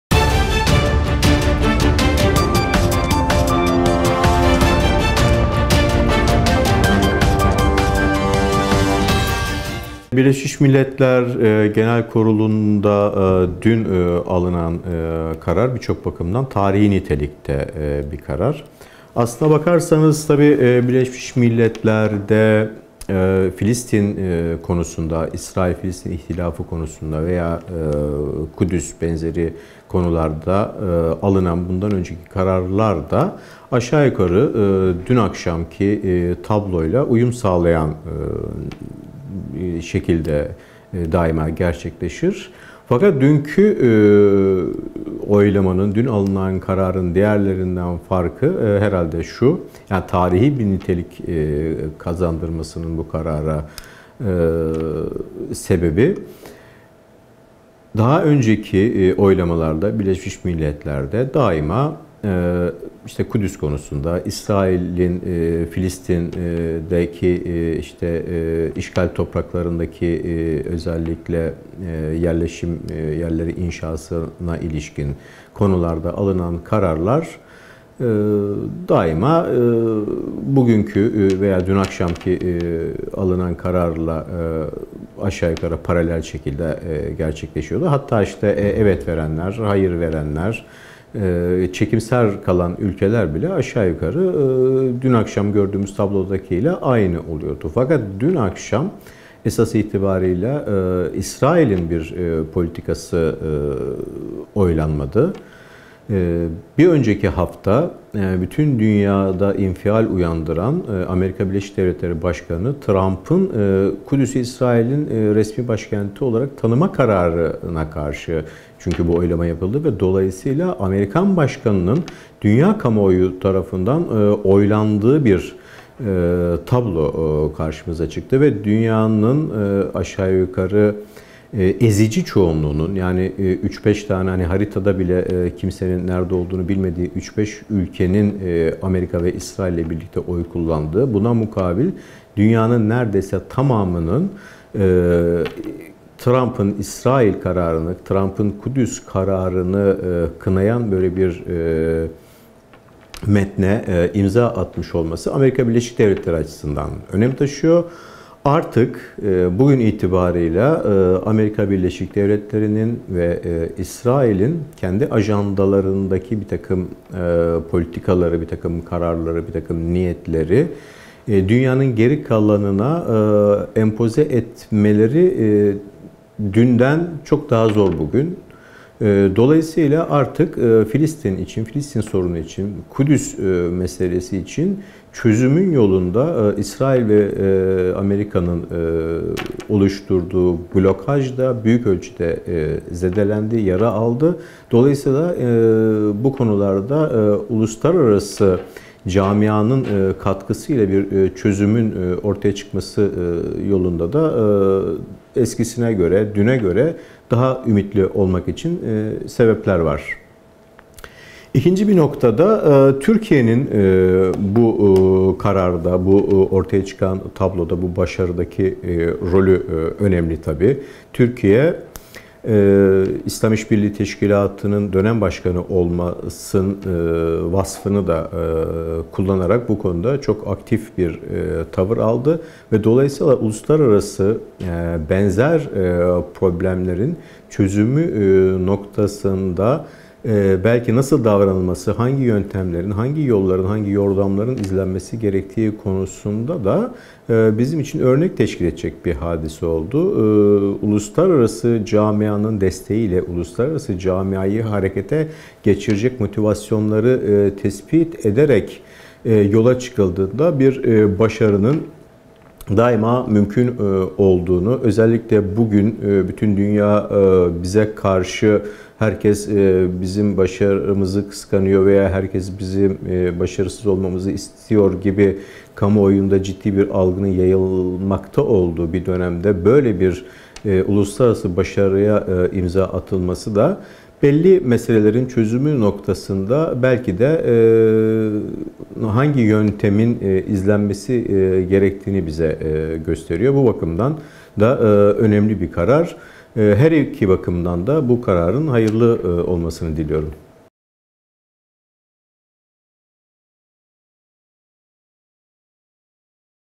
İbrahim Kiras Kudüs kararını yorumluyor
Karar Gazetesi Yayın Yönetmeni İbrahim Kiras, BM'de kabul edilen Kudüs tasarısını yorumladı. Kiras, Türkiye'nin başını çektiği adımın iki açıdan tarihi nitelikte olduğunu söyledi.